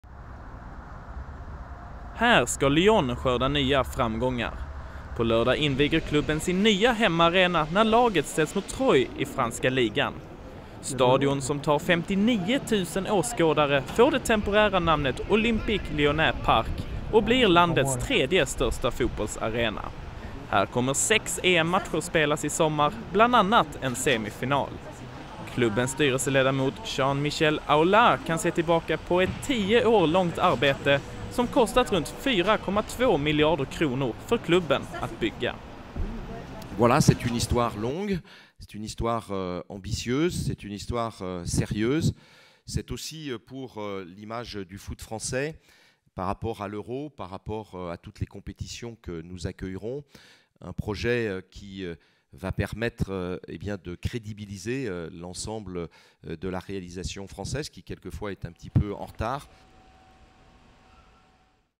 Spana in Lyons nya skrytbygge
Det tog tio år och kostade över fyra miljarder svenska kronor - men nu är Lyons nya hemmaarena klar och redo för invigningsmatch. Till sommaren spelas dessutom sex EM-matcher på arenan som tar 59 000 åskådare.